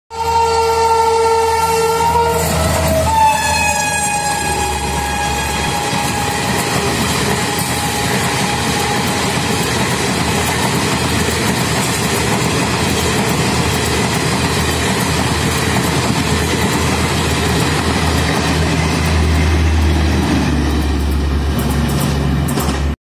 12878 Ranchi Garib Rath skipping Jeonathpur.
12878 New Delhi - Ranchi Garib Rath skipping Jeonathpur and overtaking 12322 Howrah Mail.